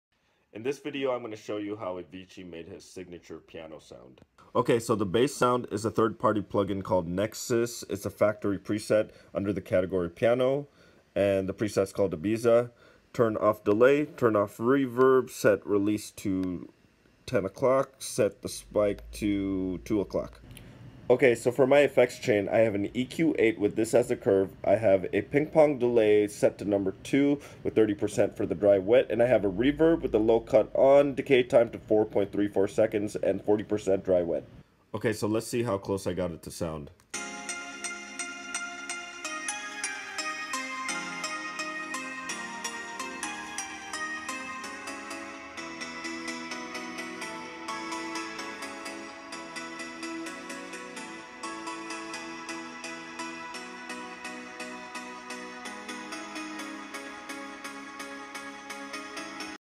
Avicii's Signature Piano Sound 1 Minute Tutorial
Whenever an artist I like passes away, I get into this habit of studying and analyzing their music, and not simply just listening to his/her songs. I feel you get a unique perspective into their creative mind when you look at their notations and instrument settings. I was up late in bed studying Avicii's signature piano sound that is present or layered in many of his famous songs. I did a quick flash tutorial just to show how simple it is to achieve this. My goal is not to enable everyone to copy the sound but to help inspire folks to expand on it. This tutorial is dedicated to the memory of Avicii.